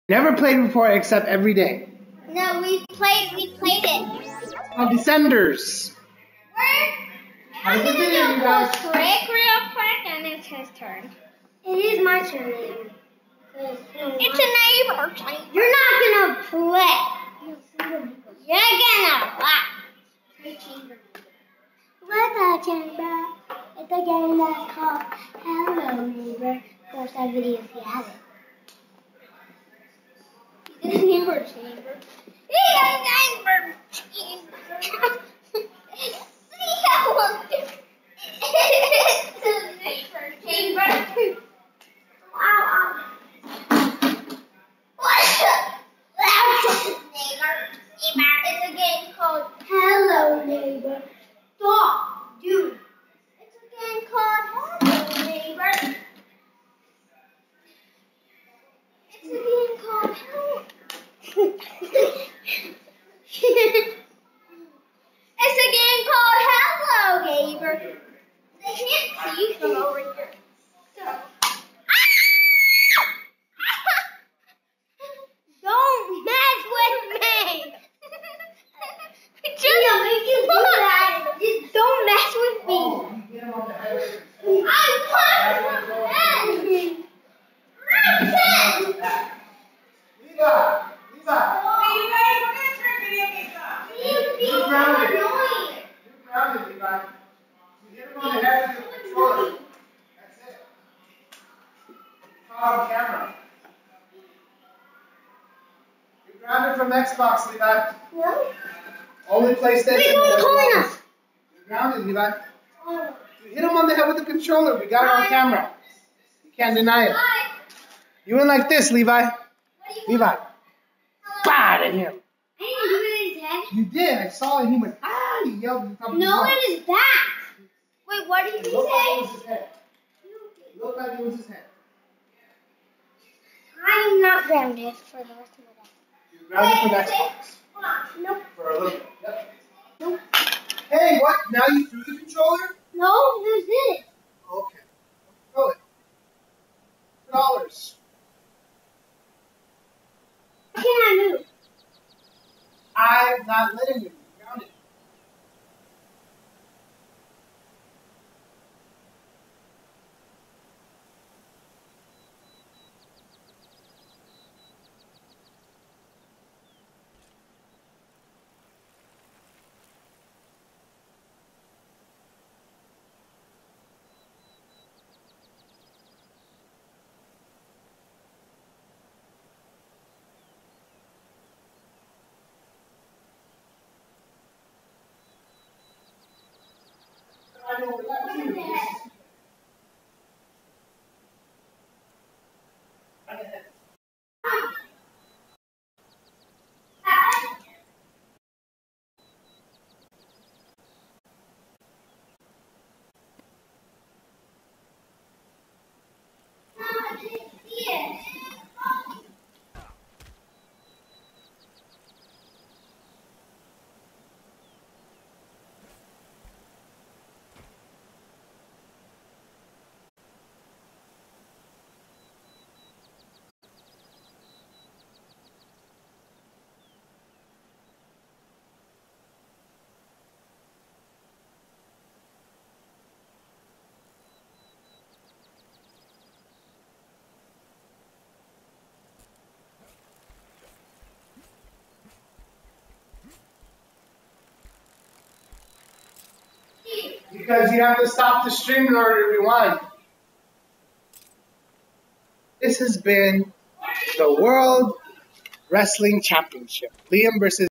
Liam Vs Levi: THE REVENGE
Levi hits Liam in the head with the controller.